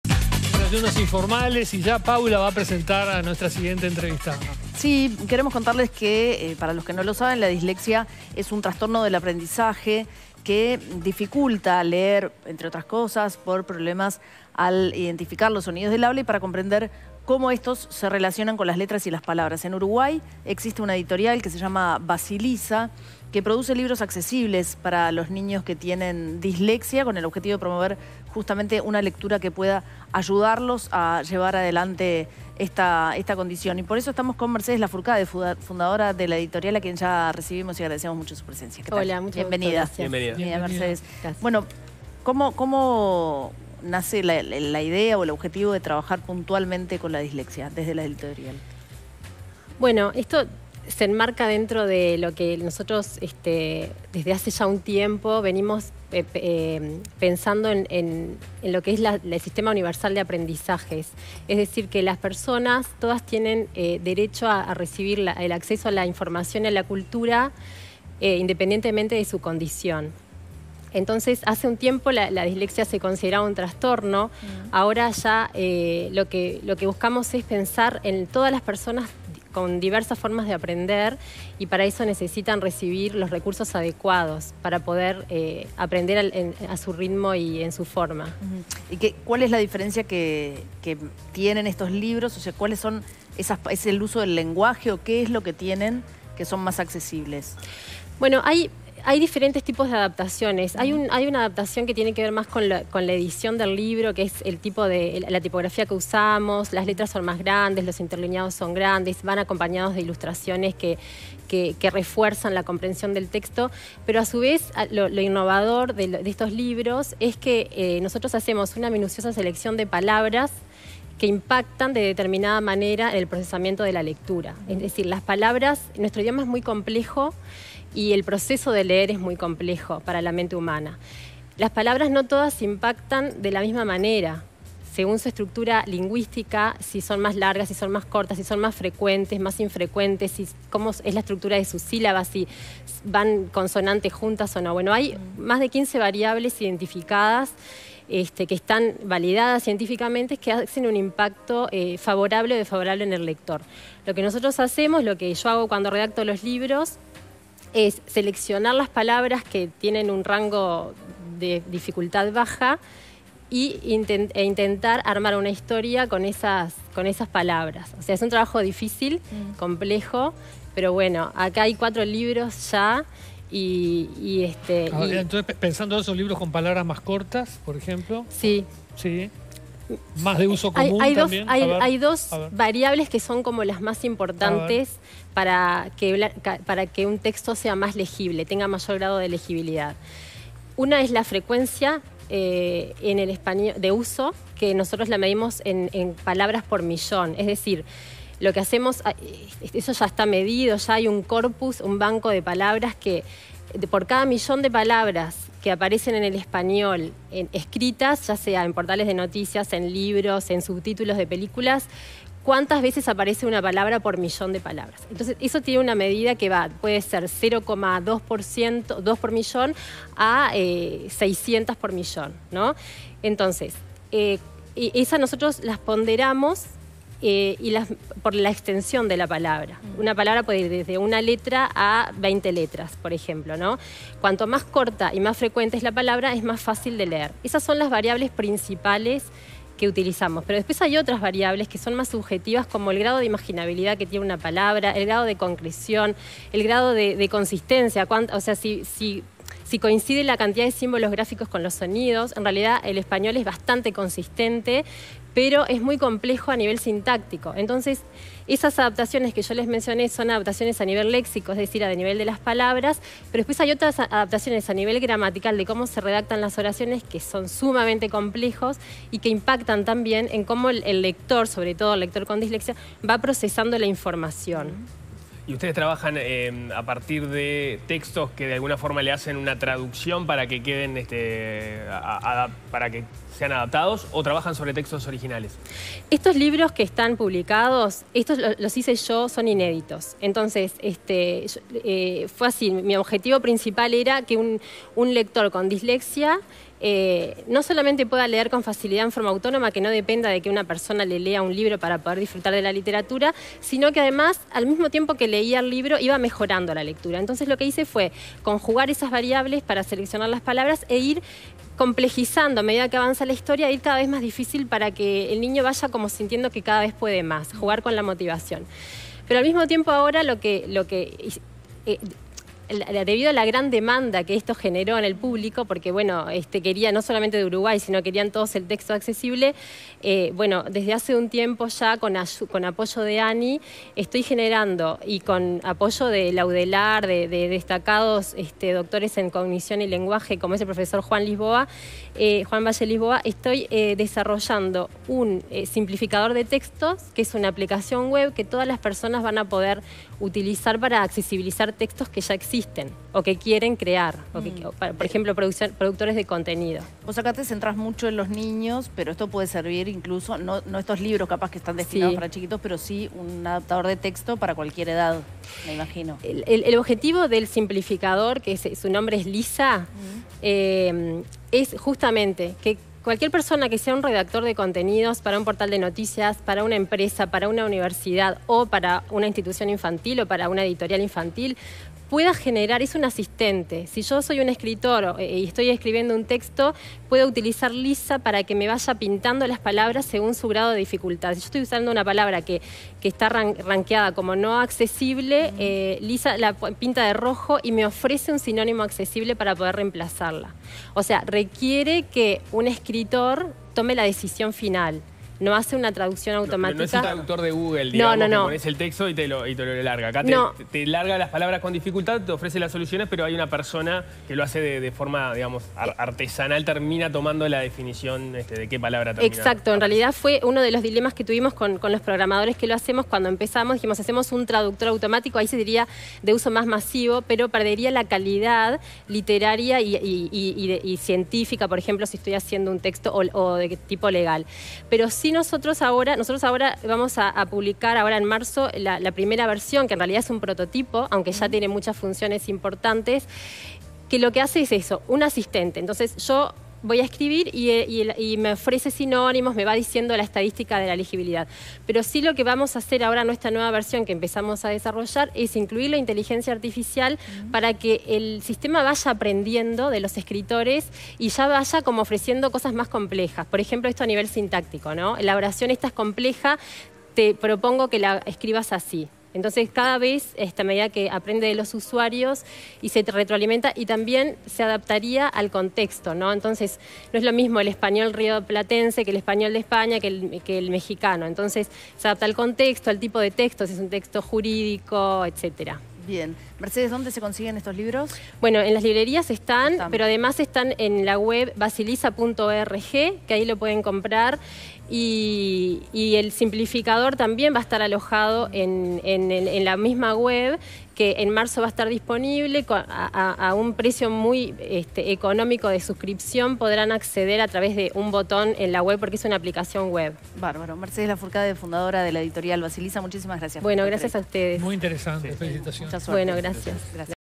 Basilisa: Una editorial que busca "facilitar y mejorar la lectura autónoma" para niños con dislexia
La dislexia es un trastorno del aprendizaje que supone la dificultad para leer a raíz de problemas para identificar los sonidos del habla y para comprender cómo estos se relacionan con las letras y las palabras. En Uruguay, existe una editorial conocida como Basilisa -fundada por Mercedes Lafourcade- que produce libros accesibles para los niños que tienen esta enfermedad, con el objetivo de promover una lectura adaptada a su problema.